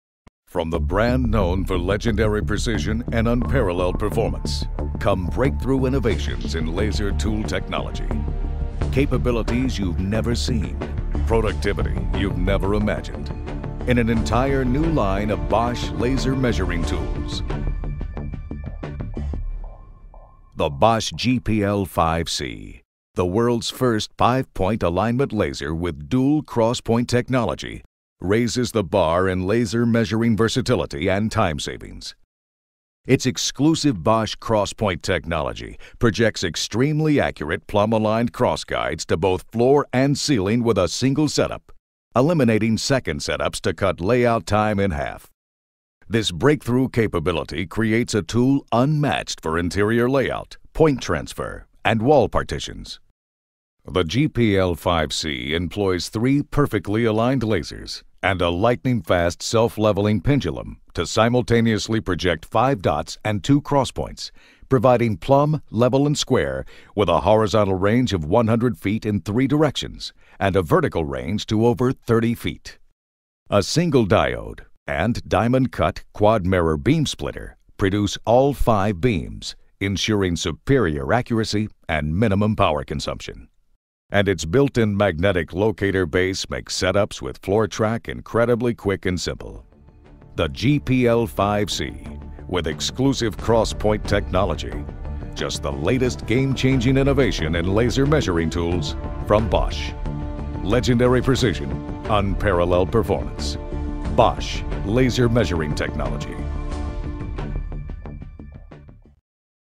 Bosch GPL 5 C Punktlaser
Bosch GPL 5 C Professional är en robust punktlaser med upp till 4° lutning. Punktlasern har skyddsklass IP54 och är perfekt för användning på byggarbetsplatser. Punktlasern är lättanvänd tack vare självförklarande funktionsknappar för olika projektionsmönster. Den har arbetsintervall upp till 30 m med nivelleringsnoggrannhet på ± 0,2 mm/m horisontalt.